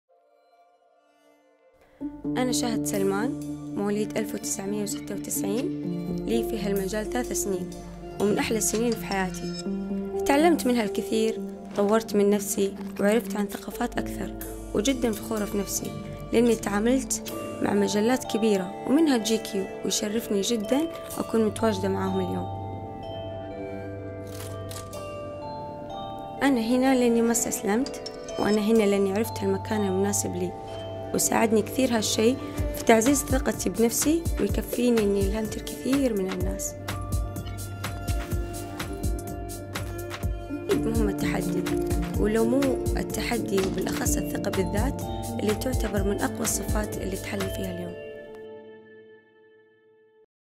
Shahad Salman Is Defying Expectation | GQ Middle East
Shahad Salman was more than inspired by the global success of the model Winnie Harlow. This wasn’t even about her own success or failure, it was about representation. The simple fact that somebody like her, with vitiligo, was in magazines and on billboards, became a living and breathing example that her dreams were possible.

Our six-part series on Arabs changing the cultural landscape all over the world continues with Shahad Salman